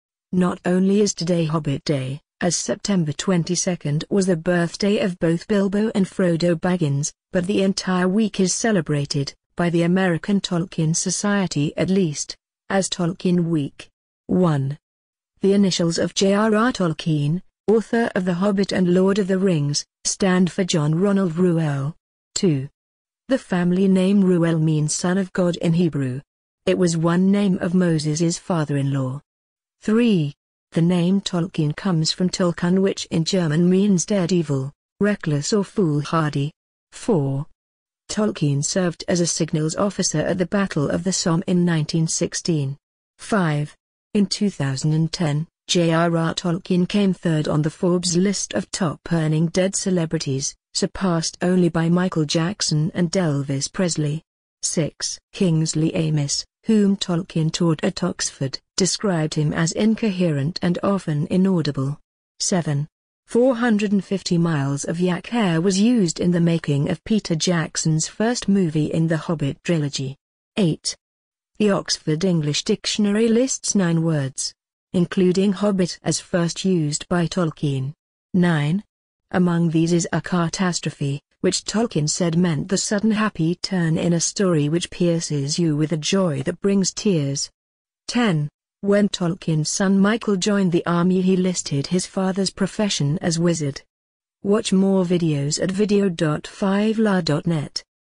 Top 10 facts about JRR Tolkien
Not only is today Hobbit Day (as September 22 was the birthday of both Bilbo and Frodo Baggins) but the entire week is celebrated (by the American TolkienSociety at least) as Tolkien Week.
1. The initials of JRR Tolkien, author of The Hobbit and Lord Of The Rings, stand for John Ronald Reuel.
2. The family name Reuel means “son of God” in Hebrew. It was one name of Moses’s father-in-law.
3. The name Tolkien comes from “tollkühn” which in German means daredevil, reckless or foolhardy.
4. Tolkien served as a signals officer at the Battle of the Somme in 1916.
5. In 2010, JRR Tolkien came third on the Forbes list of Top-Earning Dead Celebrities, surpassed only by Michael Jackson and Elvis Presley.
6. Kingsley Amis, whom Tolkien taught at Oxford, described him as “incoherent and often inaudible”.
7. 450 miles of yak hair was used in the making of Peter Jackson’s first movie in the Hobbit trilogy.
8. The Oxford English Dictionary lists nine words, including “hobbit” as first used by Tolkien.
9. Among these is “eucatastrophe”, which Tolkien said meant “the sudden happy turn in a story which pierces you with a joy that brings tears”.
10. When Tolkien’s son Michael joined the army he listed his father’s profession as “wizard”.
WILLIAM HARTSTON

👉 Overweight people 'hold key' to tackling diabetes, acco